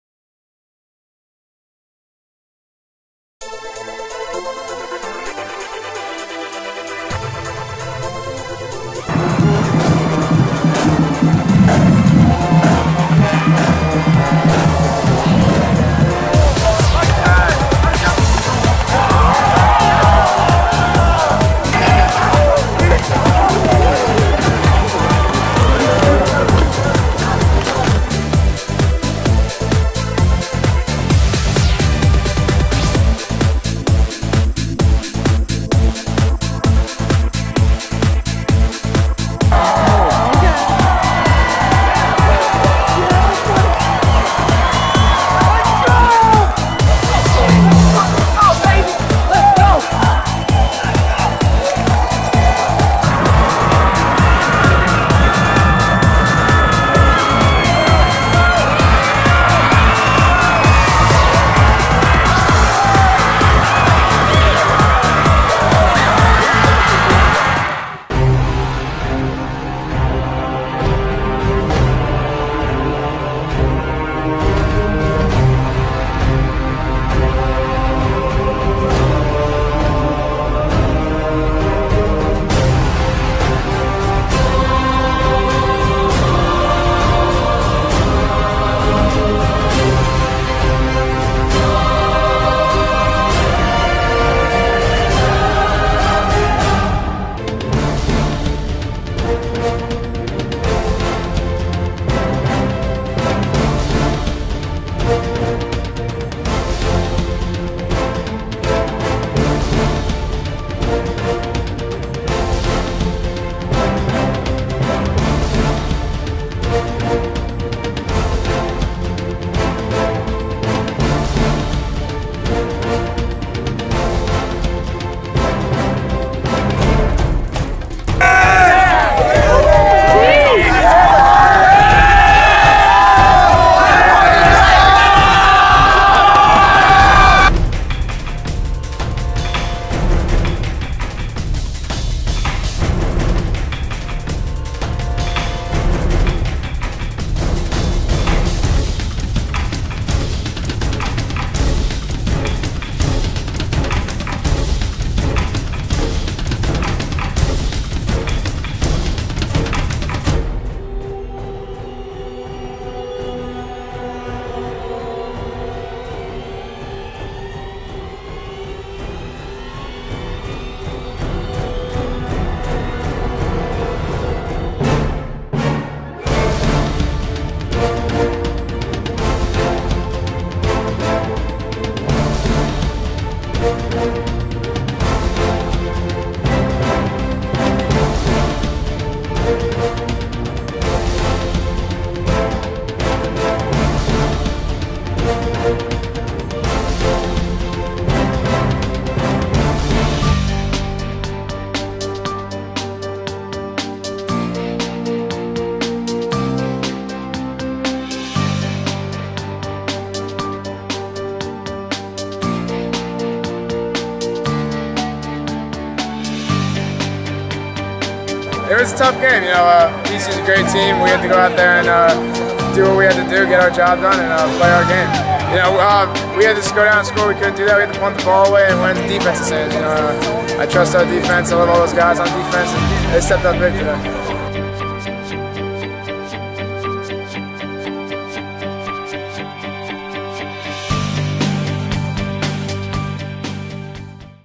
Bosco @ PC 10 19
DBP @ Paramus Catholic Friday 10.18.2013  Final Score Bosco 20 PC 13